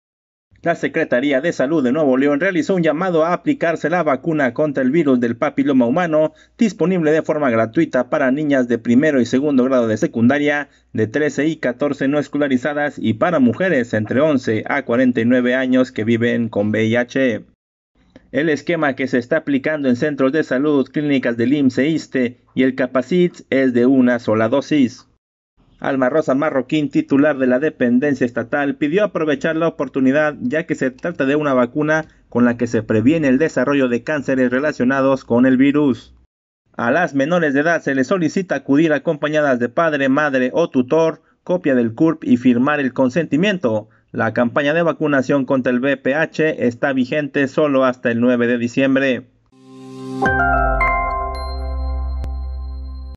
Llaman a prevenir VPH
La Secretaría de Salud de Nuevo León realizó un llamado a aplicarse la vacuna contra el Virus del Papiloma Humano (VPH), disponible de forma gratuita para niñas de 1° y 2° grado de secundaria, de 13 y 14 años no escolarizadas y para mujeres entre 11 a 49 años que viven con VIH.